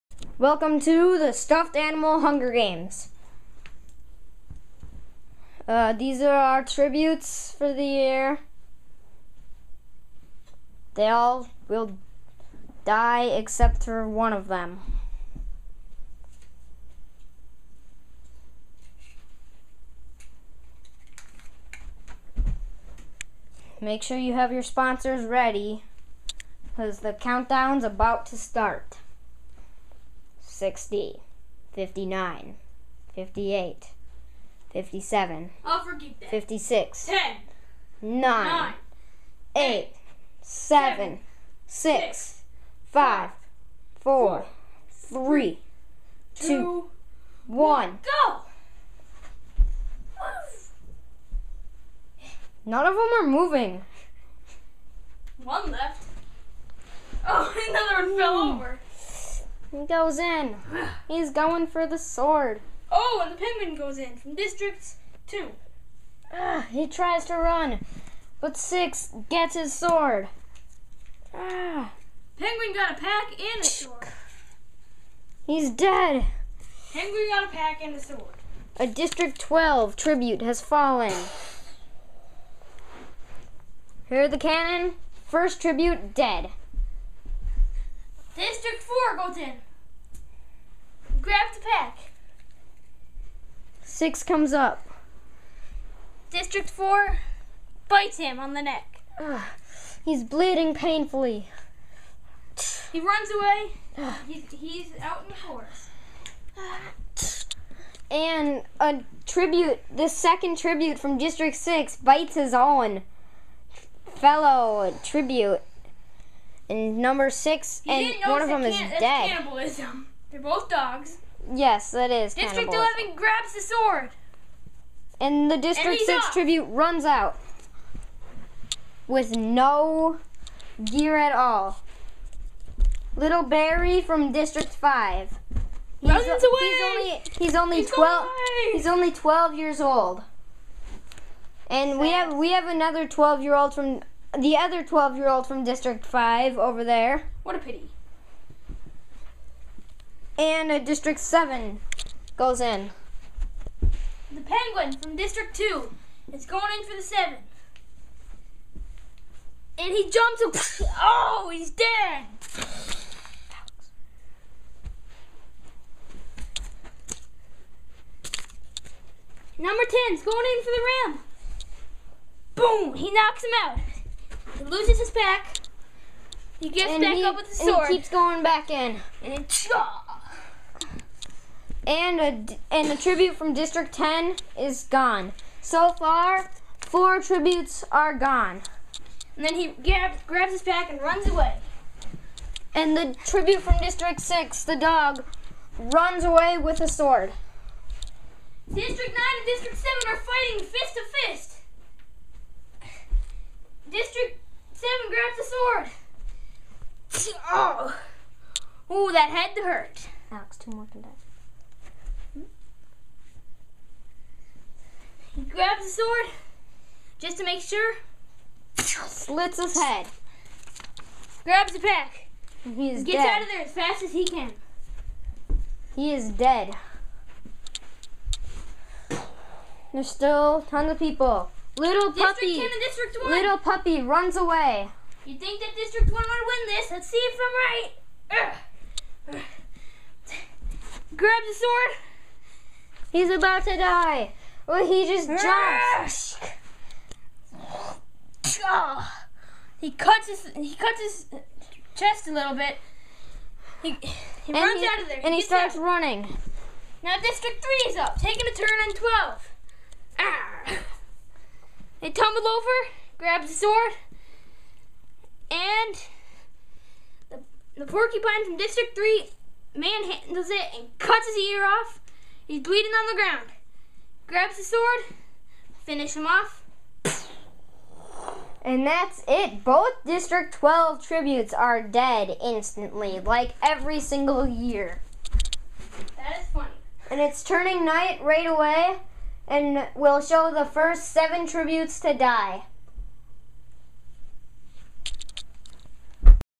Stuffed Animal Hunger Games Ep.1 The Games Begin!
Sorry for the poor footage. It's hard to control the tributes and the camera. :P